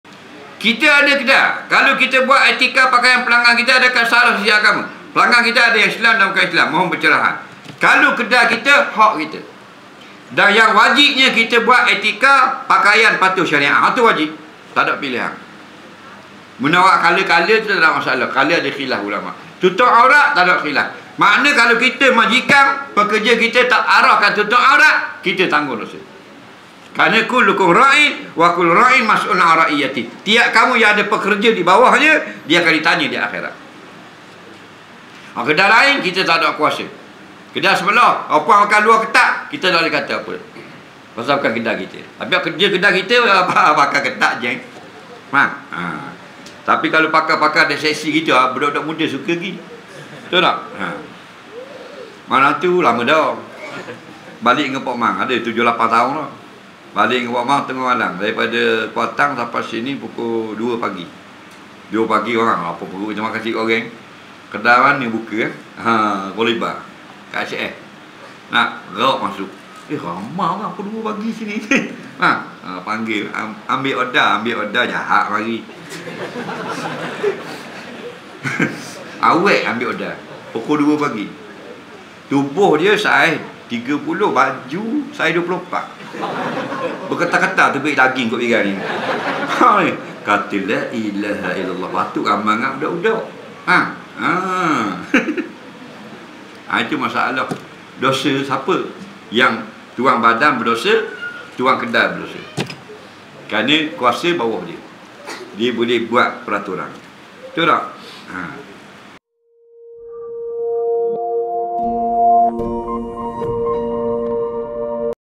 Buat Etika Pemakaian Kepada Pelanggan Kedai Kita - Ustaz Azhar Idrus
📆 14 Ogos 2023 | Isnin
⏰ 7.45 malam
🕌 Surau Kg Sungai Pinang, Gelugor Kedai, Terengganu.
📖 Kitab : Kuliah Jemputan